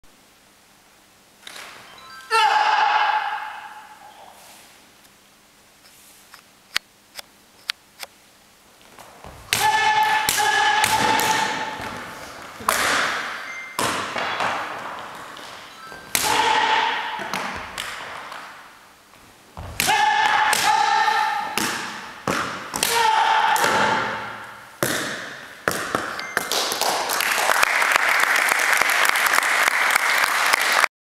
Bamboo cutting
Short bamboo cutting demonstration from Haedong Kumdo seminar in Helsinki on March 2008